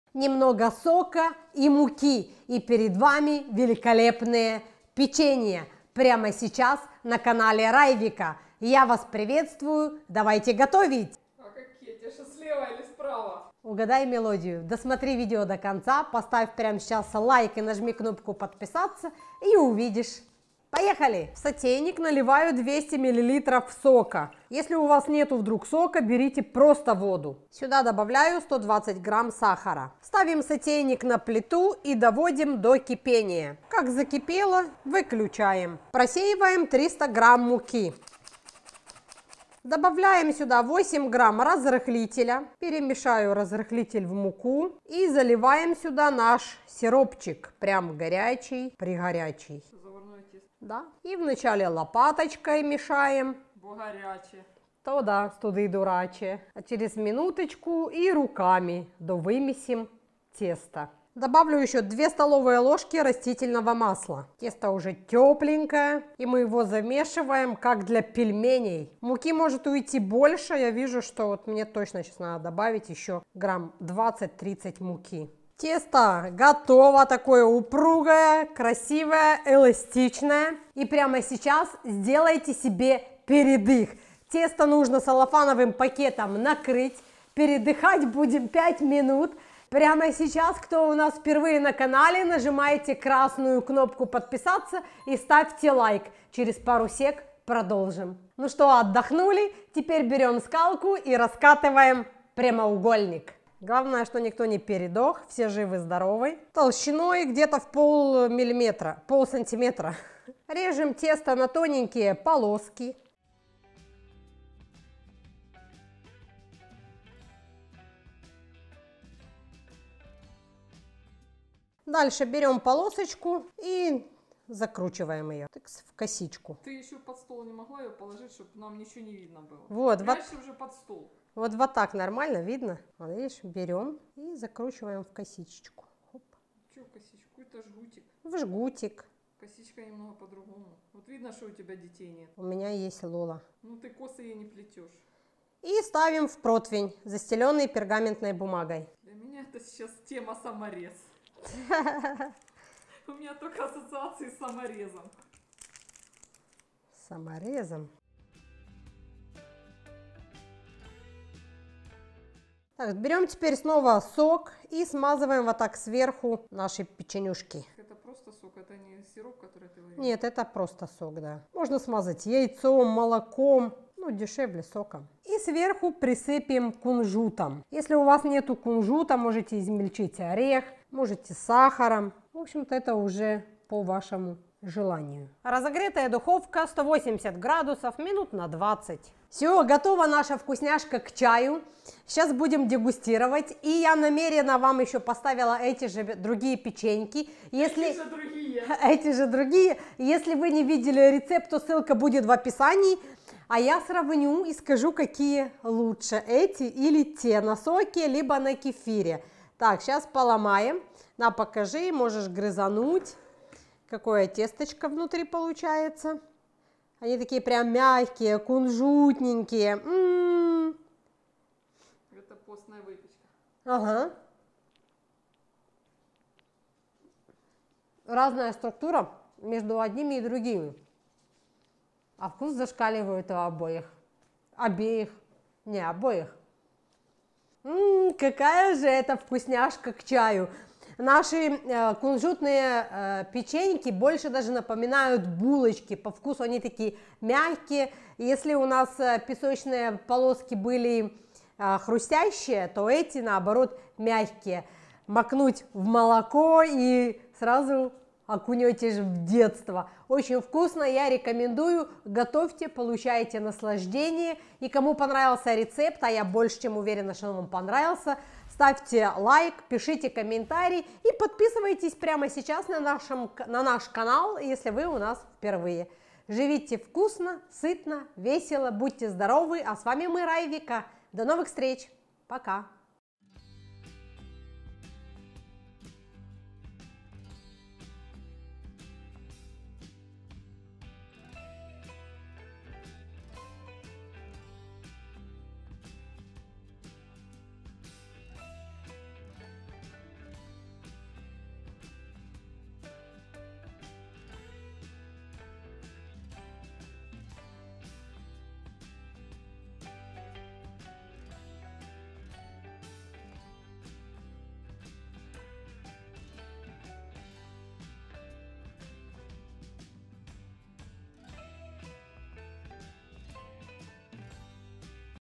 1 Стакан СОКА и 10 минут Вкуснятина на столе ПЕЧЕНЬЕ К ЧАЮ! Быстрая ВЫПЕЧКА из ДОСТУПНЫХ Продуктов.
НЕМНОГО СОКА и  НЕЖНОЕ ПЕЧЕНЬЕ у ВАС НА СТОЛЕ!  Теперь готовим постоянно к чаю только это!  Рецепт к чаю из ничего. Удивительно ВКУСНОЕ ПОСТНОЕ (ВЕГАНСКОЕ) Печенье. Кунжутное ароматное  ПЕЧЕНЬЕ на соке. Готовится из самых простых ингредиентов. Будете готовить еще и еще!

ИНГРЕДИЕНТЫ:
Мука 300 гр
Сок 200 мл